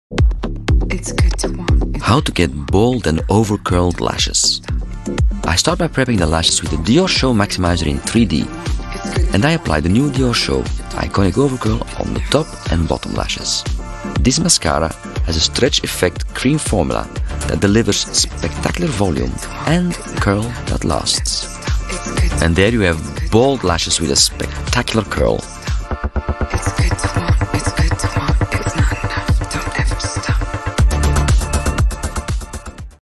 Diorshow Iconic Overcurl - Tutorial
La máscara Diorshow Iconic Overcurl se ha reinventado para dar al maquillaje de ojos un volumen y curvatura aún más espectacular.

El pincel curvo de la máscara es fácil de usar para crear pestañas de aspecto más largo y abrir los ojos.

La máscara está enriquecida con néctar de algodón para pestañas que se ven más fuertes y brillantes cada día.

El diseño moderno de la icónica máscara Dior cuenta con una banda facetada en el mismo tono: negro, azul, marrón o plata con brillo.